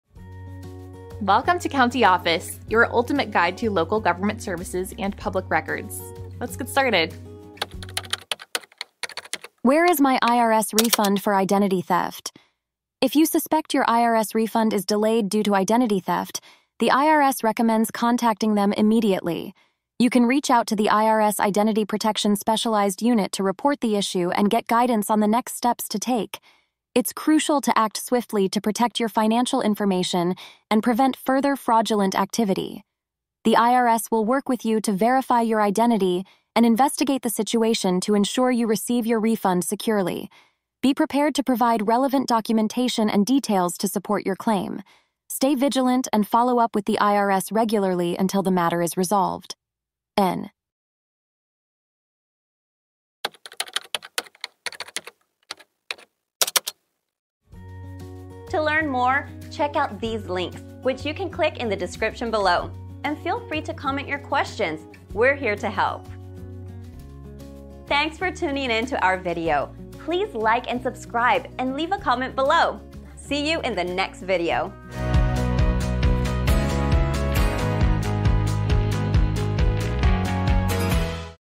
Where Is My IRS Refund for Identity Theft? - CountyOffice.org
Where Is My IRS Refund for Identity Theft? Join us for an insightful discussion on what to do if you suspect your IRS refund is delayed due to identity theft. In this video, we'll guide you on the steps to take and resources available to resolve this issue swiftly and securely. The IRS recommends immediate action to protect your financial information and prevent further fraudulent activity. Learn how to report identity theft to the IRS Identity Protection Specialized Unit and get assistance on verifying your identity. Stay informed on the necessary documentation and follow-up needed to ensure you receive your refund safely. Don't let identity theft disrupt your financial security - act now and stay vigilant until the matter is resolved.